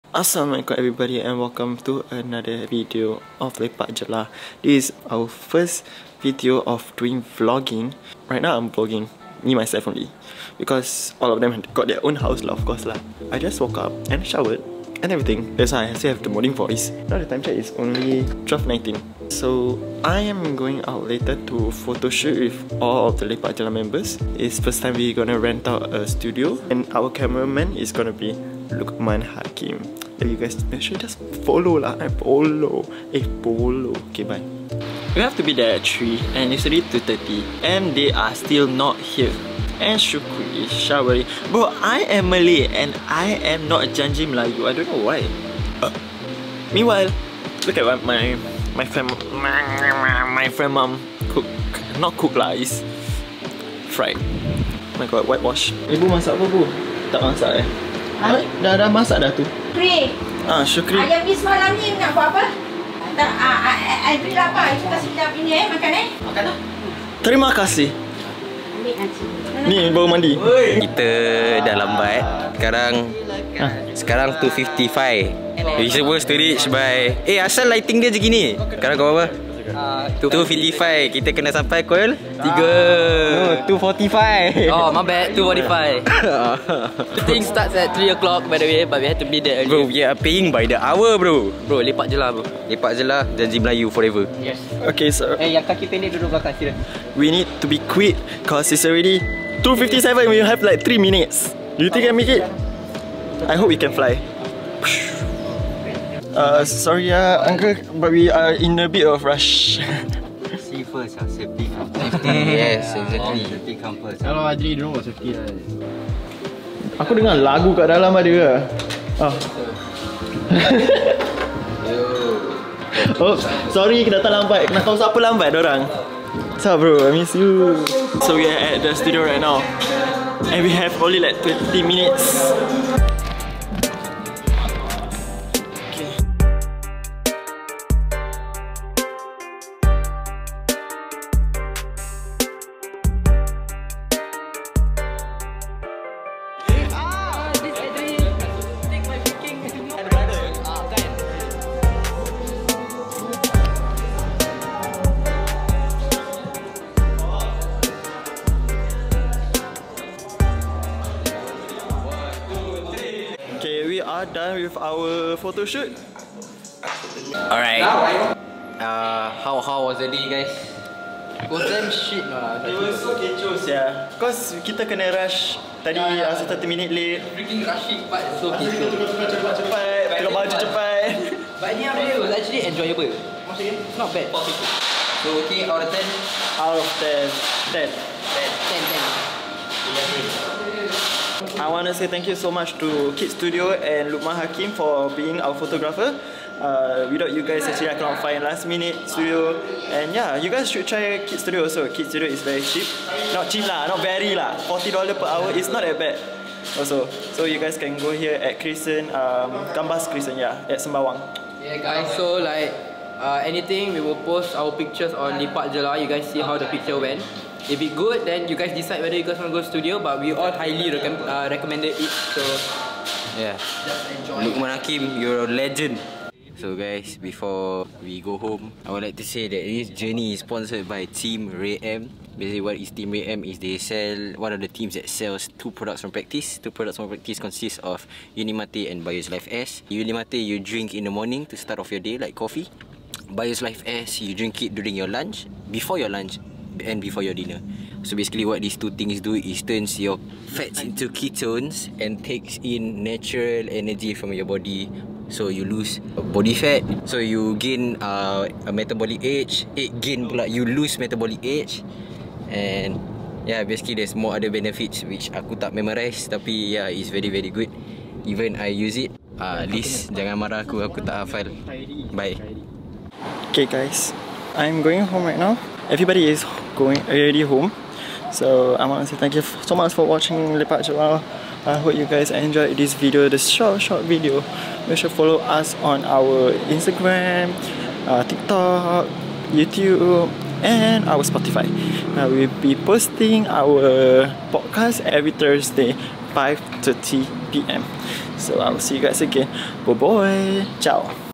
LEPAKJELAH PHOTOSHOOT! | VLOG #1
Assalamuailaikum everybody! This is our first time ever doing vlog as LEPAKJELAH! Hope you enjoy this short vlog from us and we will be doing this more!